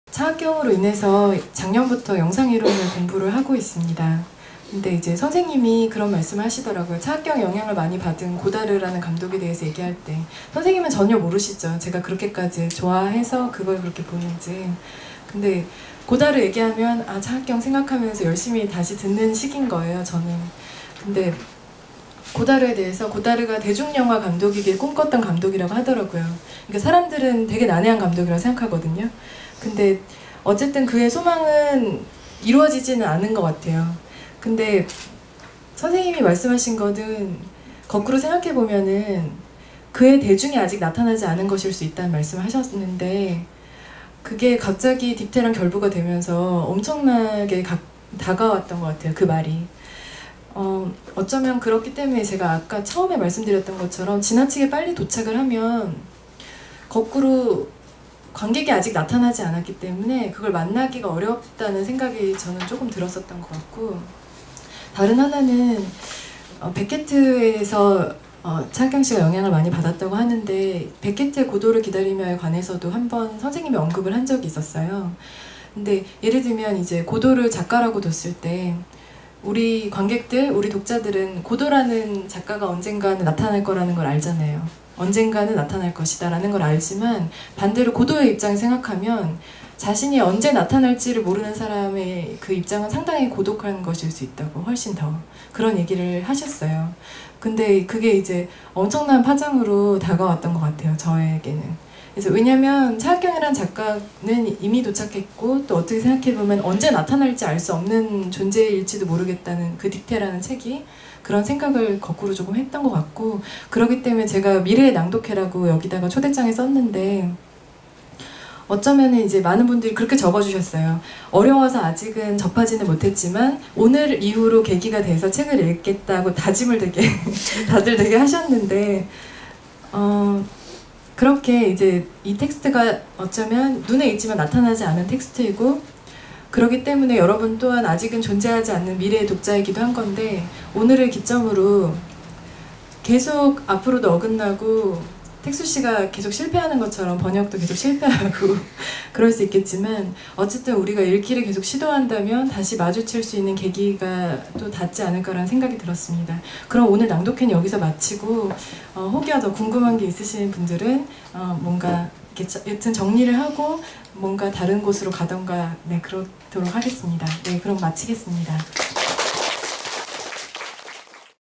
네시이십분 라디오 2주년 공개방송, 차학경 '딕테' 6. 마지막 멘트
네시이십분 라디오 2주년 공개방송은 차학경의 '딕테'를 다뤘습니다. 장소는 통의동 더 북소사이어티. 차학은씨가 차학경씨에게 쓴 편지를 낭독. ('관객의 꿈'에 수록)

420 podcast radio's reading event about 'Dictee'(Theresa Hak kyung Cha)